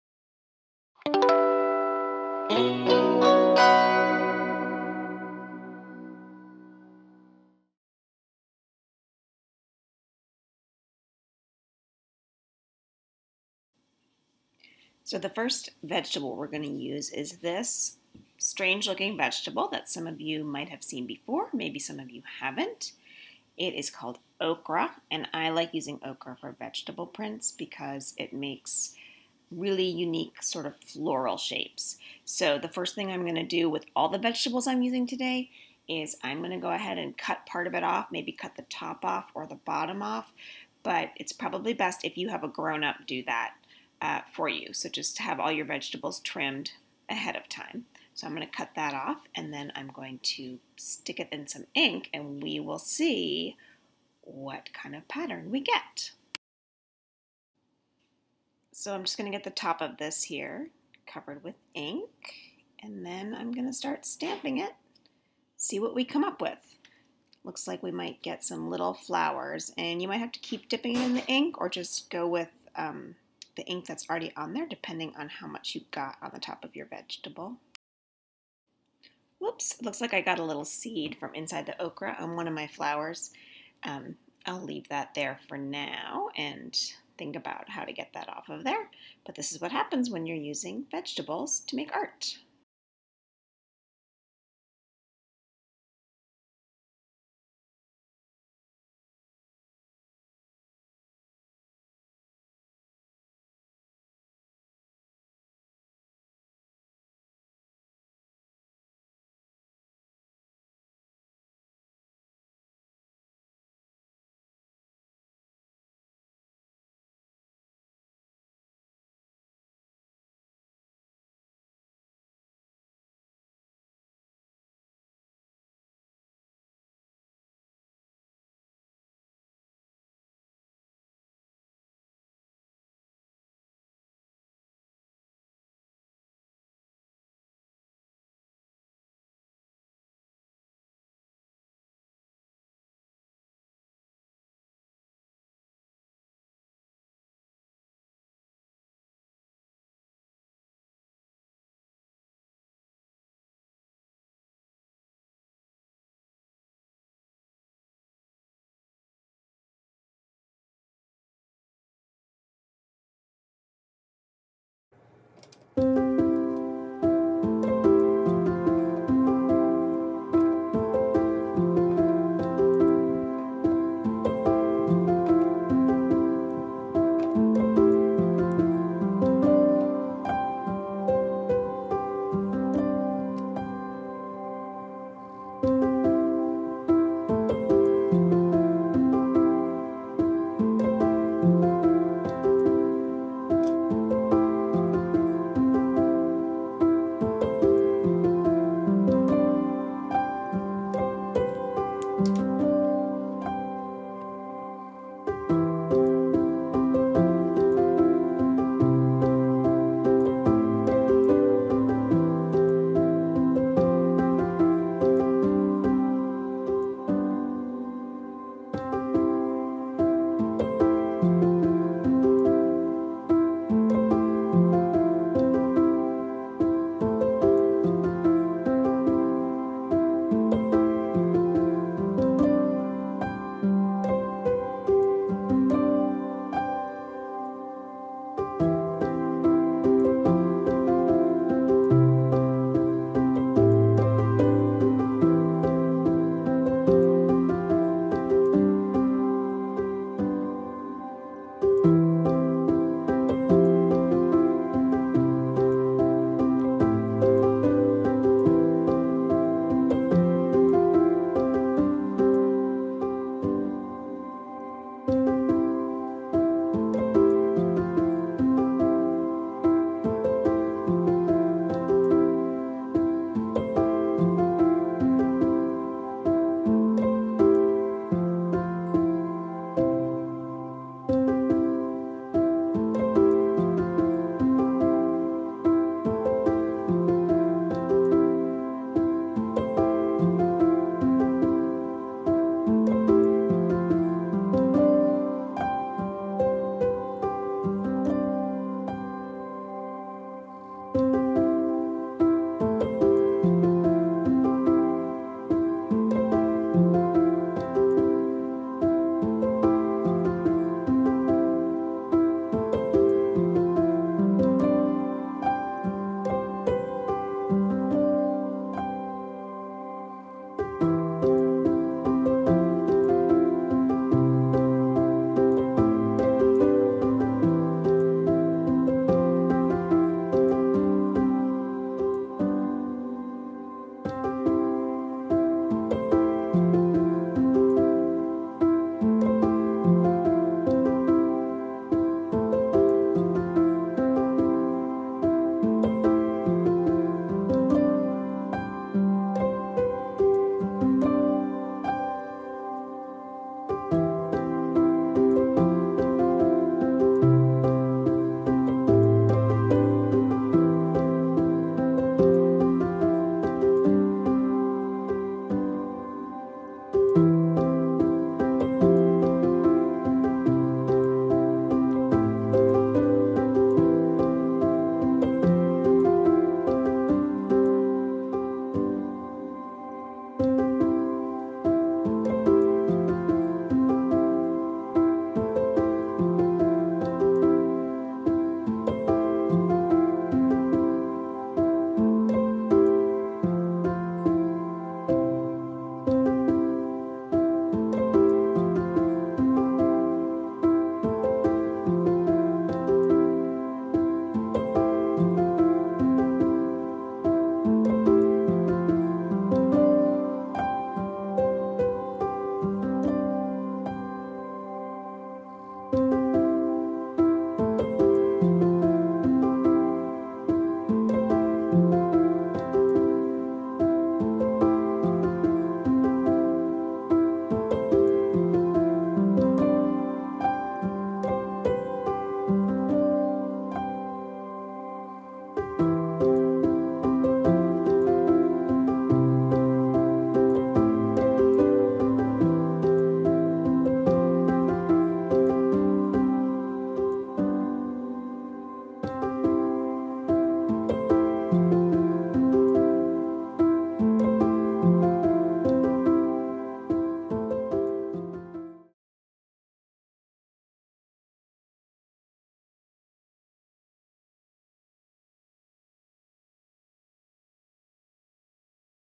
Vegetable Prints with Lucy
SUMMERGAME2020: OKRAFLOWERS Maybe you've made potato stamps before, but have you ever made flowers from okra? Clouds from broccoli? Blooms from celery? Join Lucy to see what kind of prints you can create with the vegetables in your kitchen!